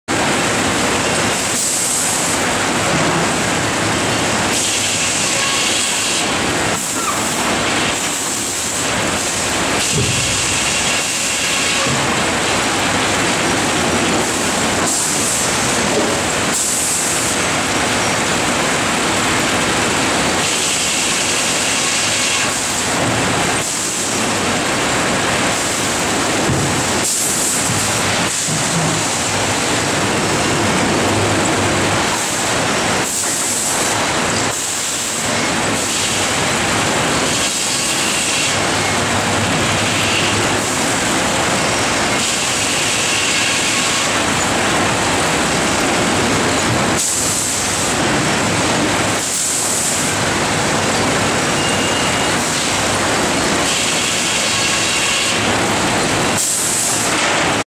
Roach Sawmill Scanning Carriage by Paw-Taw-John Services, Inc.
3 Knee Scanning Carriage Setworks Systems at Roach Sawmill in Tennessee